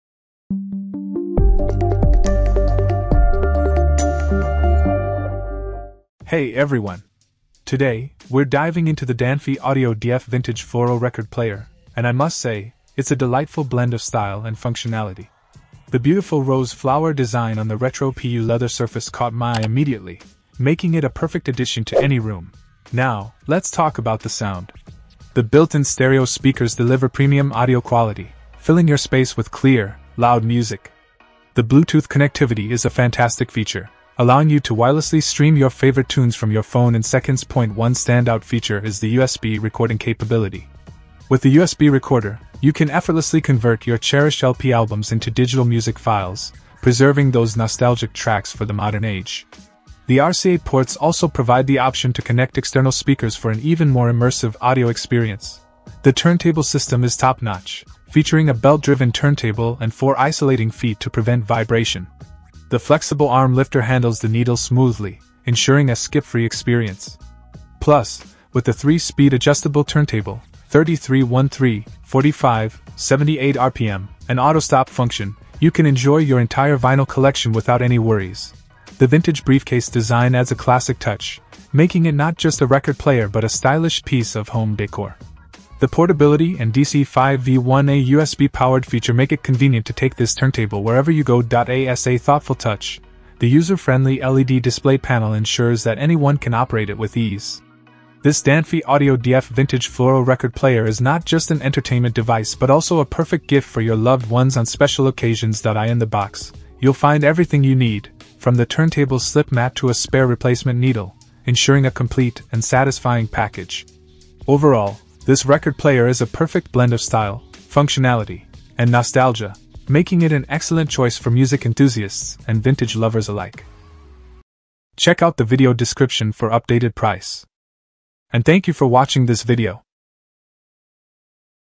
Unboxing and Review of the DANFI AUDIO DF Vintage Floral Record Player!
0:00 Intro
0:06 Review

Things that we mentioned in this video:
Record Player with Built-in : B0CD396HBX